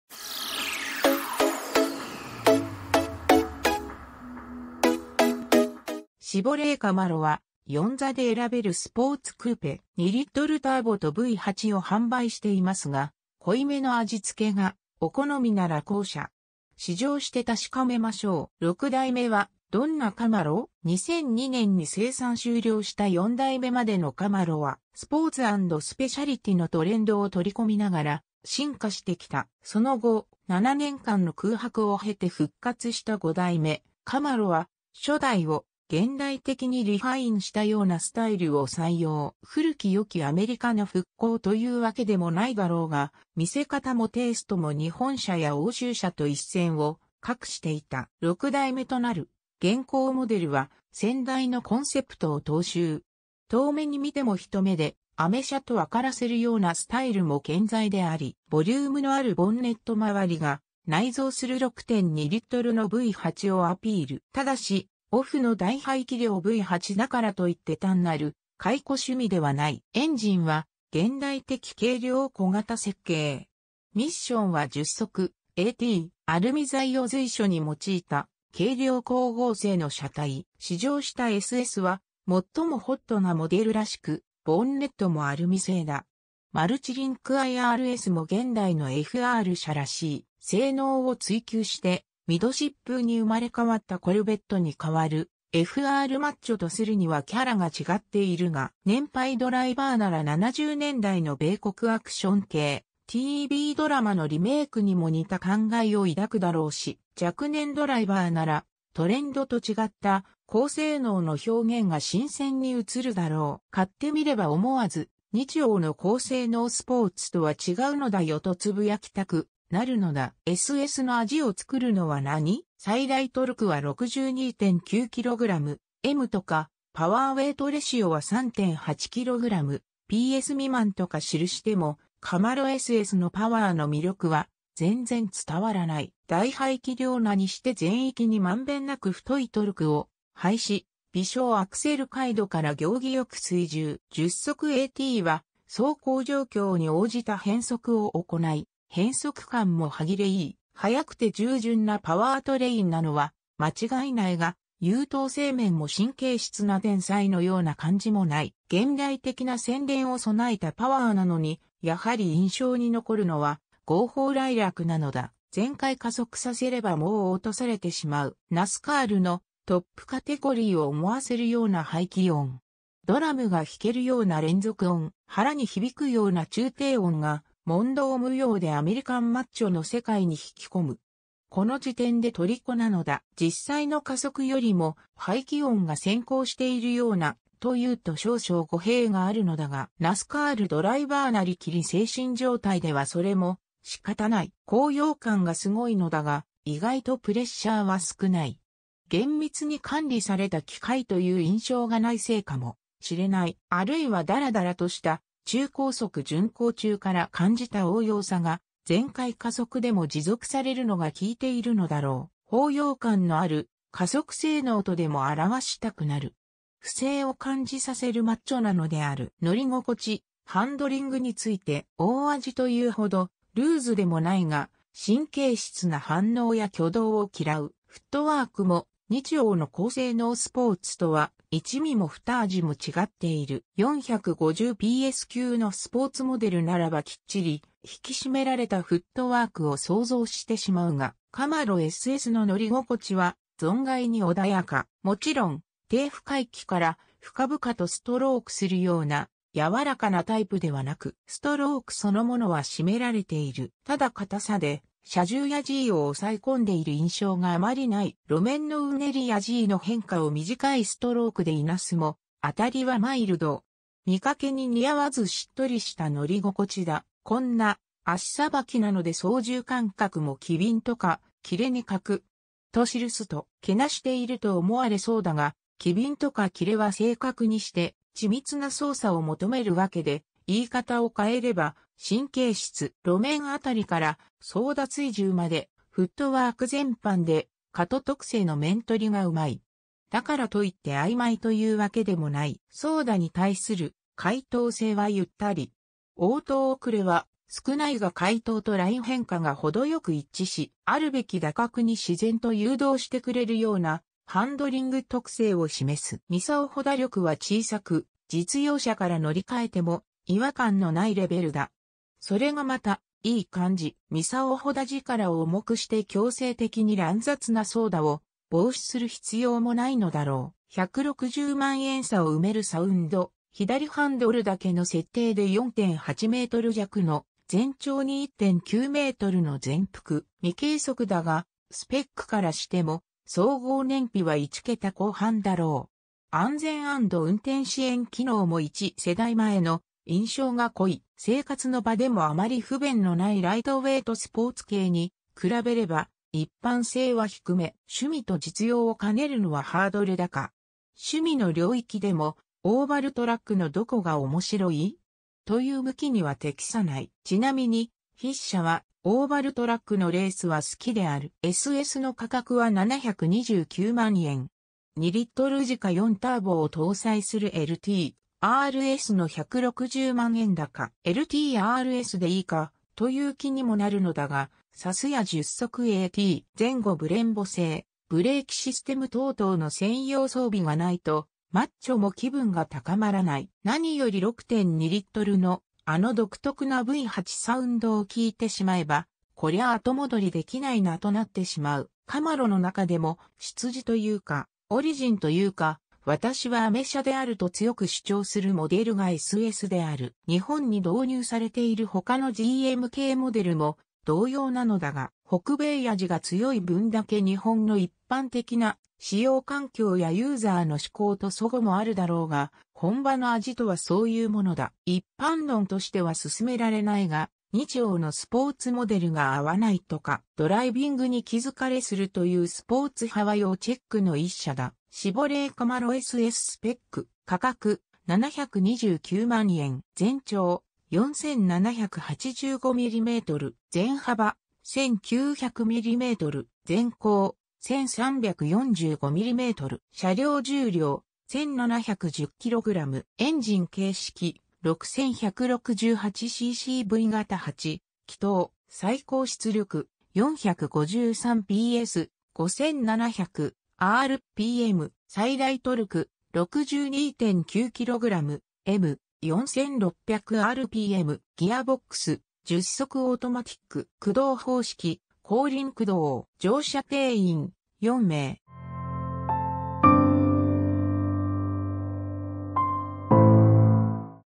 シボレー・カマロSS 大排気量V8がジワる 2022年でも虜になるアメリカン4シーター・スポーツカー | 車の話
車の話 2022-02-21

 シボレー・カマロは、4座で選べるスポーツ・クーペ。  2LターボとV8を販売していますが、濃いめの味つけがお好みなら後者。試乗して確かめましょう。  6代目は、どんなカマロ？  2002年に生産終了した4代目までのカマロは、  スポーツ＆スペシャリティのトレンドを取り込みながら進化してきた。  その後、7年間の空白を経て復活した5代目カマロは、初代を現代的にリファインしたようなスタイルを採用。  「古き良きアメリカの復興」というわけでもないだろうが、見せ方もテイストも日本車や欧州車と一線を画していた。  6代目となる現行モデルは先代のコンセプトを踏襲。遠目に見ても一目で「アメ車」と分からせるようなスタイルも健在であり、  ボリュームのあるボンネット周りが、内蔵する6.2LのV8をアピール。  ただし、OHVの